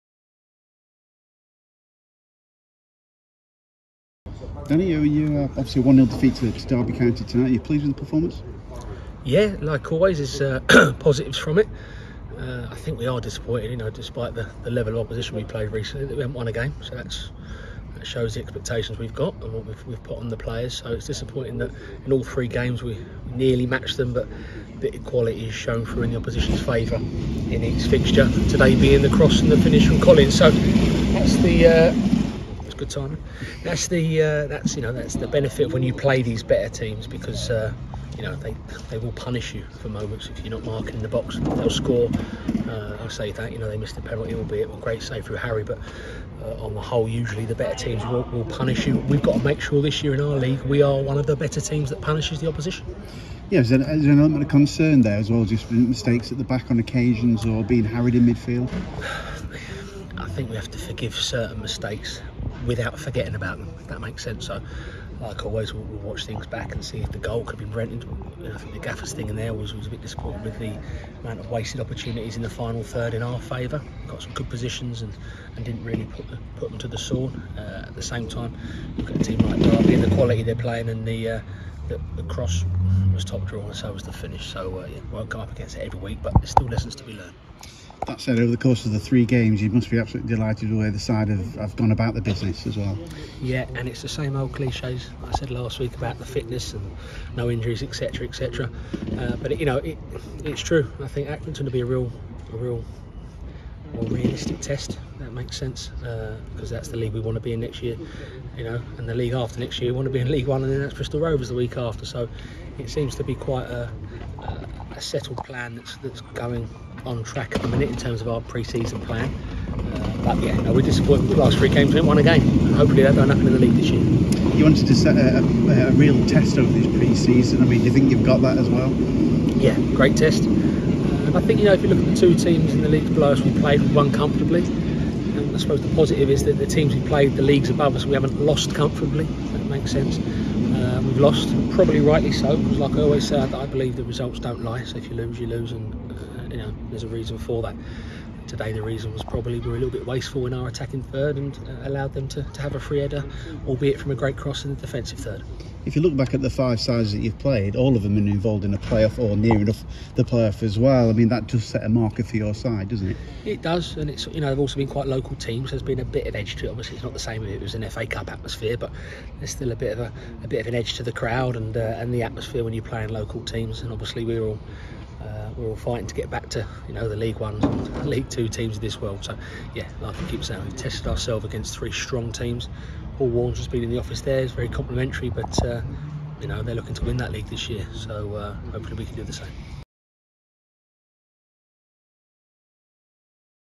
INTERVIEW | Danny Webb post-Derby County
Danny Webb spoke to the media following the Spireites' slender home pre-season defeat to Derby County.